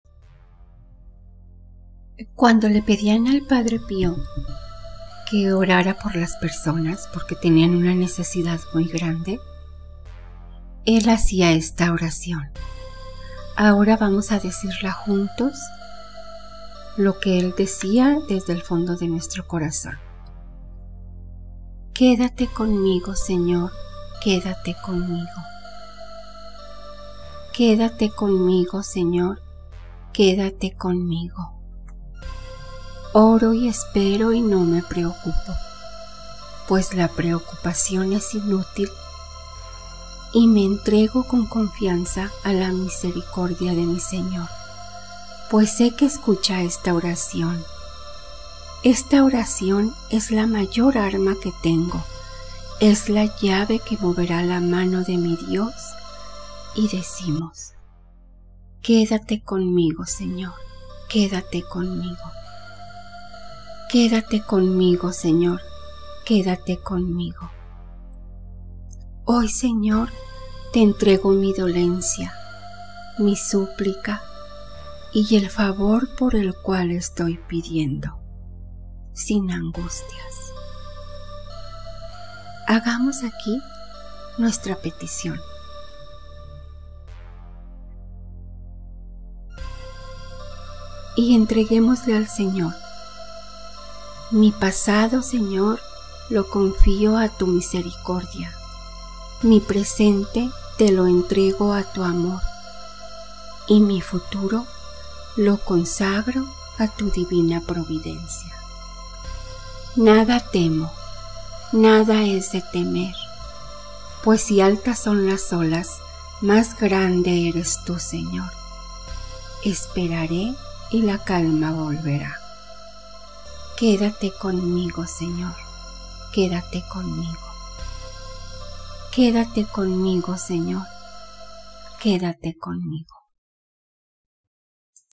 ORACION MILAGROSA DEL PADRE PIO.
CUANDO ACUDIAN AL PADRE PIO PARA QUE PIDIERA POR LAS PERSONAS HACIA ESTA ORACION. ENTREGANDOLE  TODO Y CONFIANDO EN EL SEÑOR.